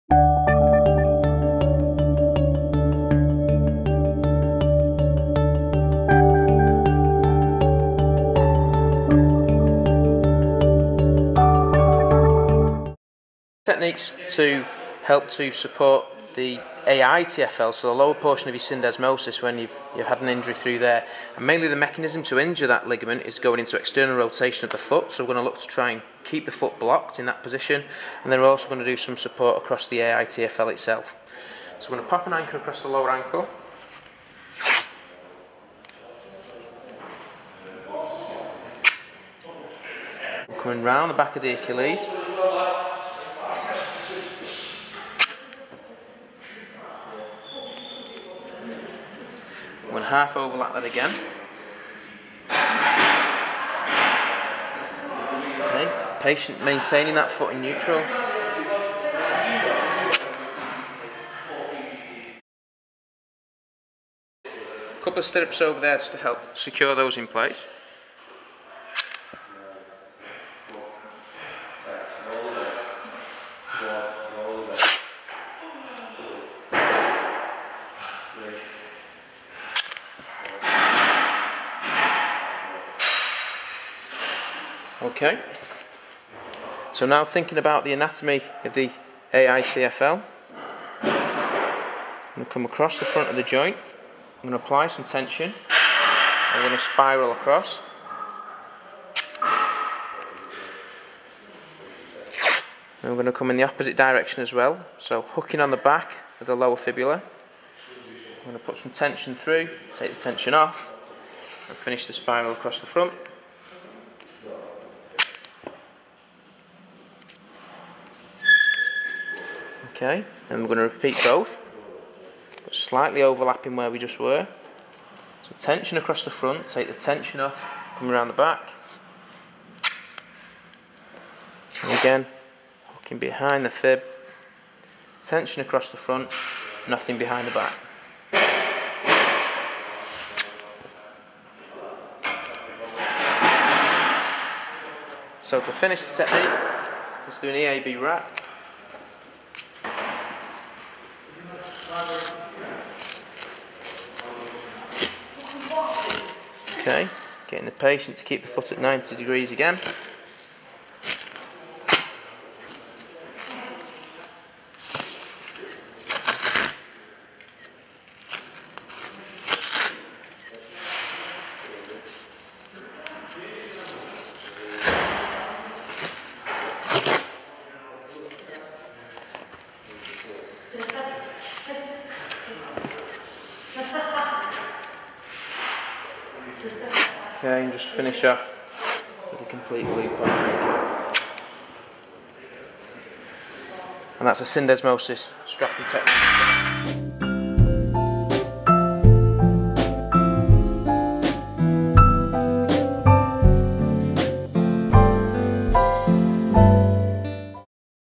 How to tape a high ankle sprain - syndesmosis sprain / support / ligament
A taping technique to support a high ankle sprain - syndesmosis of the ankle. athletic / football / rugby / hockey / strapping / physiotherapy